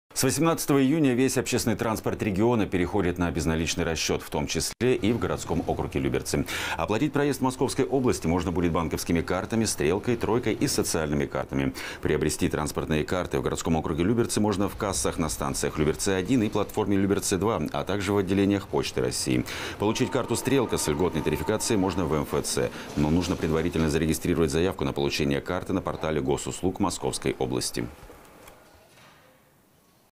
Общественный транспорт полностью переходит на безналичную оплату проезда
С 18 июня весь общественный транспорт региона переходит на безналичный расчет, в том числе и в городском округе Люберцы.
Оплатить проезд в Московской области можно будет банковскими картами, «Стрелкой», «Тройкой» и социальными картами.  Приобрести транспортные карты в городском округе Люберцы можно в кассах на станции «Люберцы-1» и платформе «Люберцы-2», а также в отделениях «Почты России».  Получить карту «Стрелка» с льготной тарификацией можно в МФЦ.  Но нужно предварительно зарегистрировать заявку на получение карты на портале госуслуг Московской области.